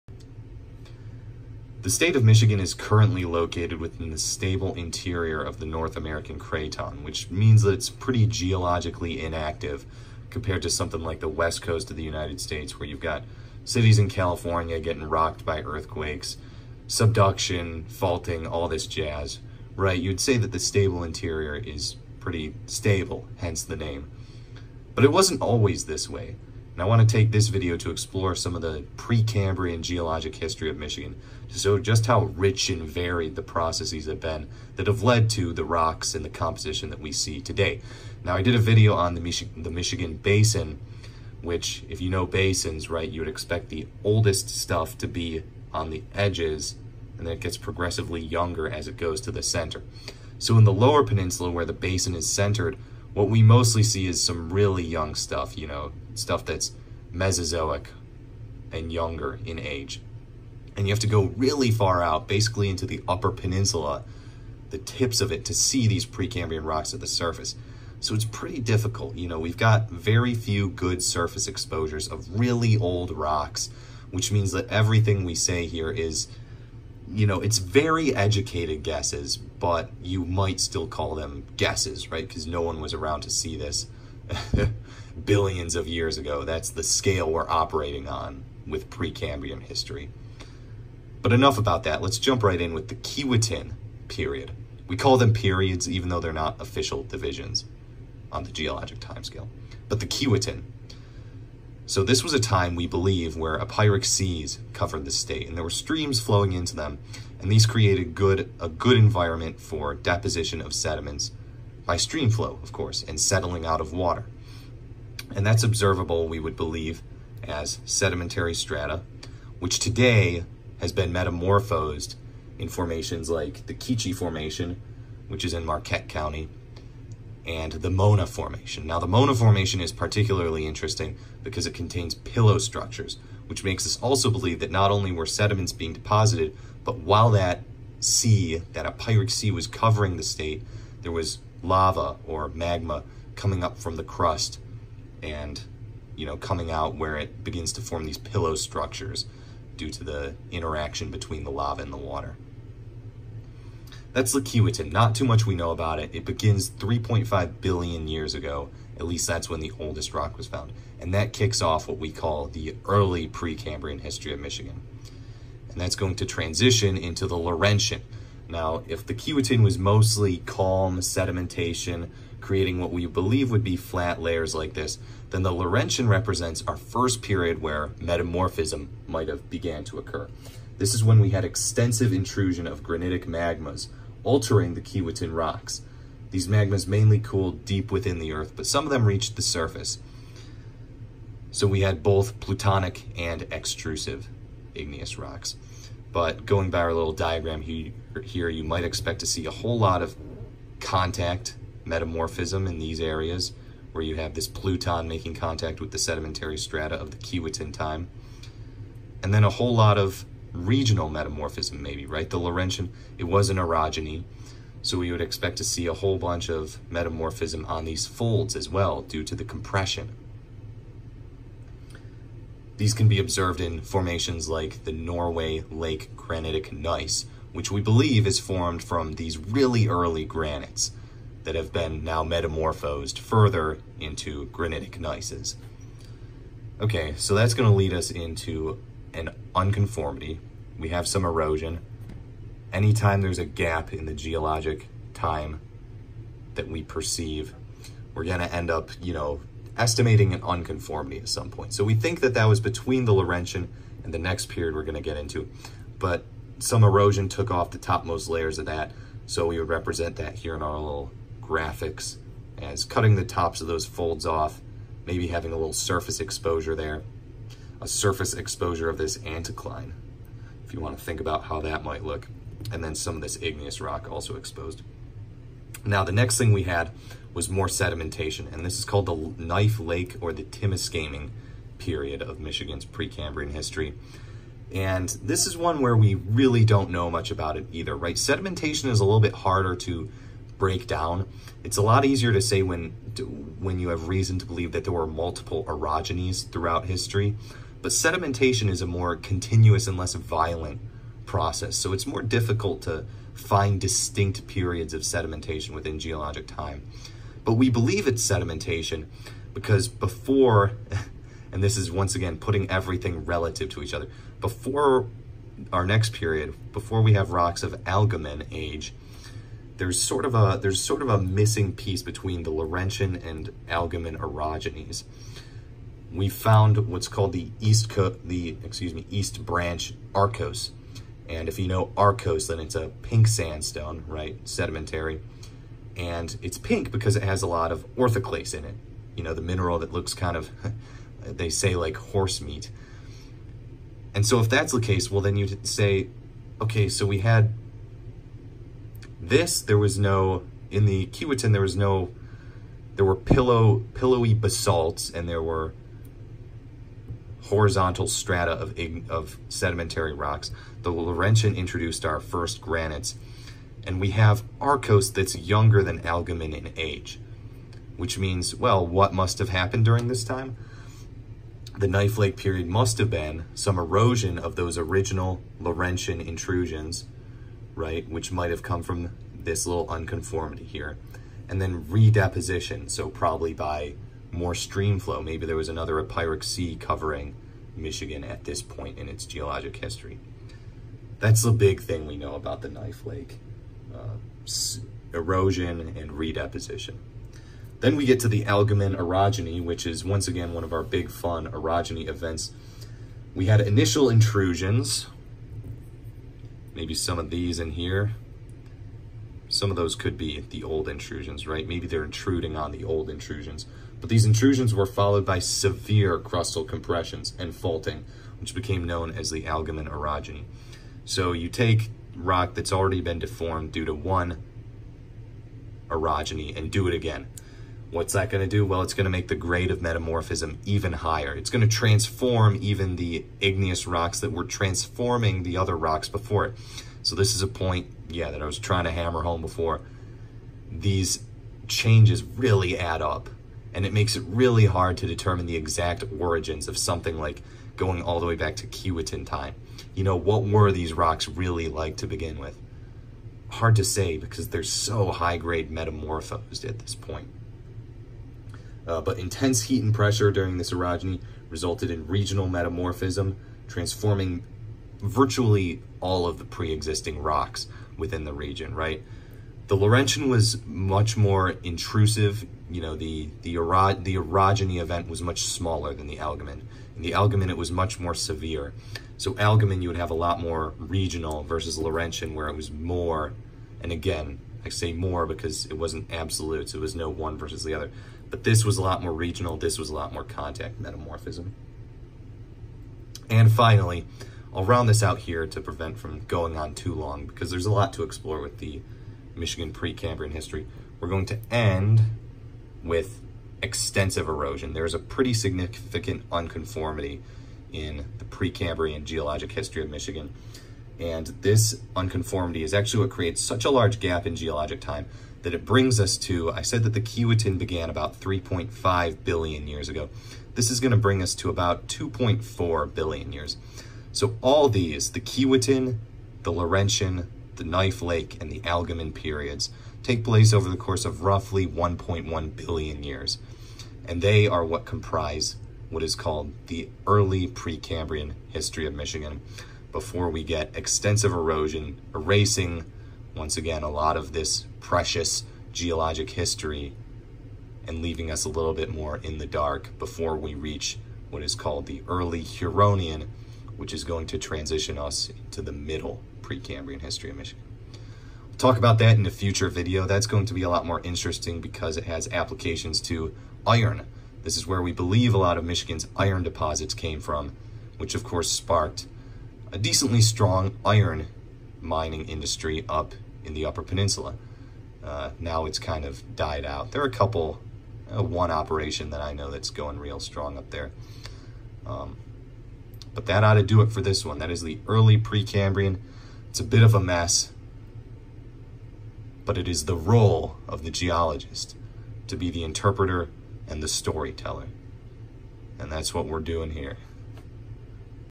Early Precambrian Geology of Michigan
A brief exploration of the Keewatin, Laurentian, Knife Lake, and Algoman periods. KLKA, for short.